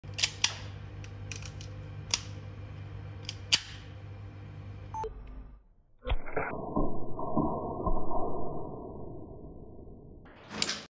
Miniature H&K MP5SD in 1/2 scale firing full-auto by MINIATUREARSENAL
100% precision and functioning Miniature H&K MP5SD in 1/2 scale firing full-auto by MINIATUREARSENAL